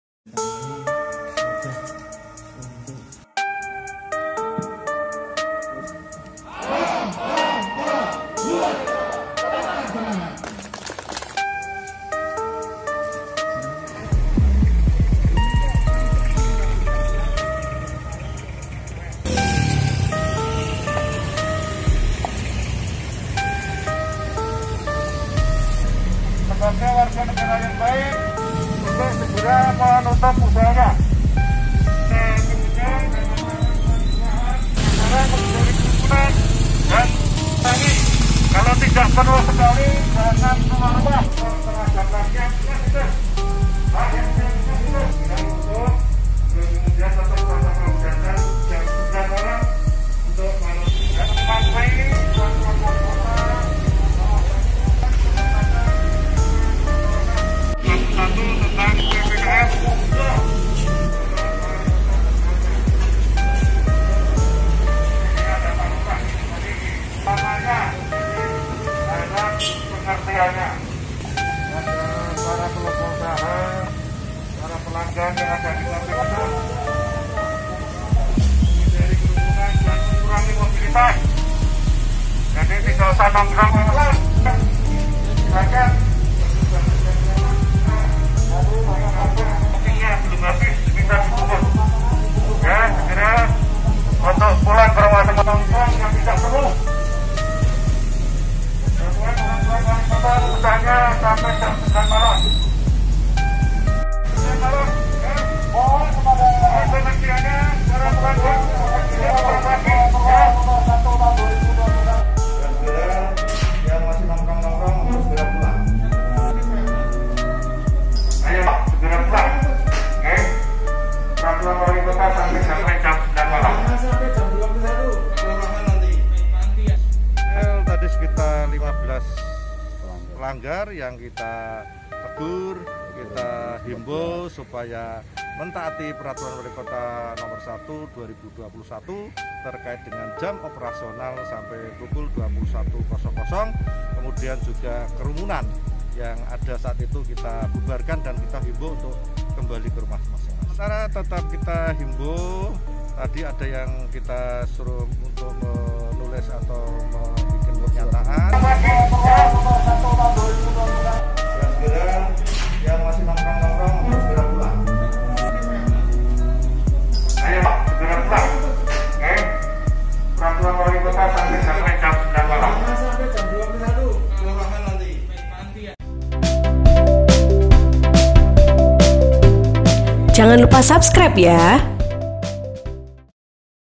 Pemilik Usaha Ngeyel Buka di Atas Jam 9 Malam Ditegur Kapolsek
Polsek Tembalang didukung jajaran Polrestabes Semarang
melakukan operasi Aman Nusa II sekaligus pantauan PPKM. Personil yang terlibat sejumlah 103 orang dari berbagai unsur. Mereka dibagi menjadi dua tim menyasar wilayah yang marak usaha PKL, angkringan, dan kafe.